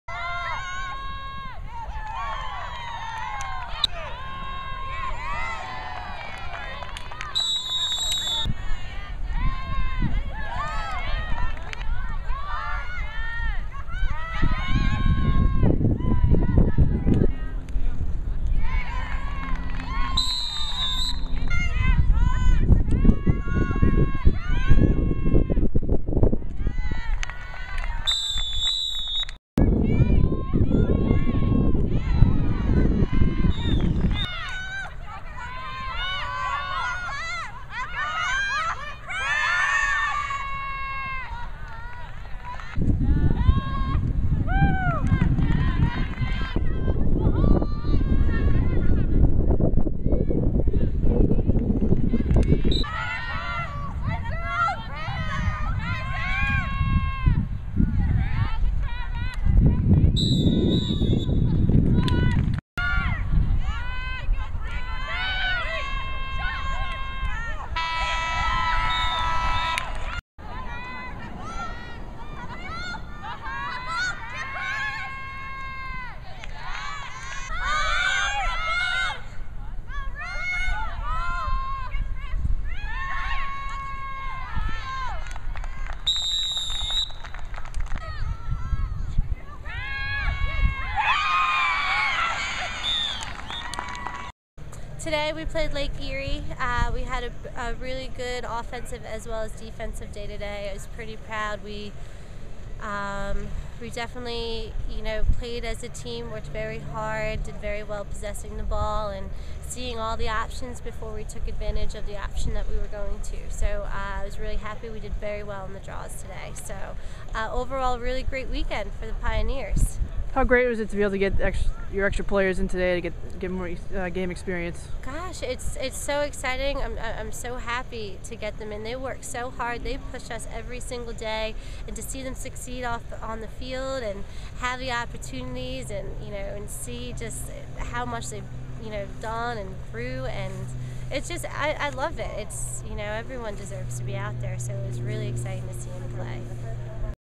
C.W. Post Women's Lacrosse vs. Lake Erie 4/1/12
C.W. Post cruised to a 23-5 conference win over Lake Erie.  Five Pioneers registered hat tricks against the Storm, including Jackie Sileo, who also had three assists.